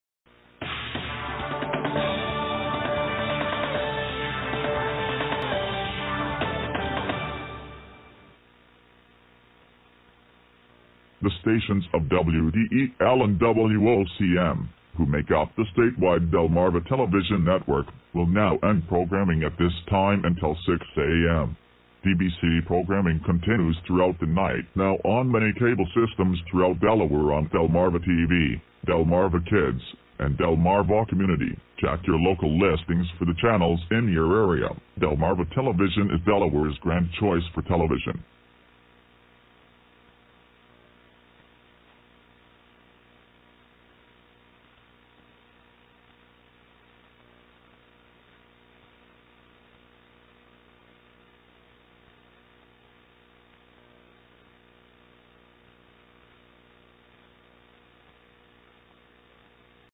Delmarva Television - Sign Off (2002)
The original video was released by a YouTuber named "DelmarvaArchiver1997" in 2009.
According to some research, this YouTuber resides in Rehoboth Beach, Maryland.
(NOTE: Fake.)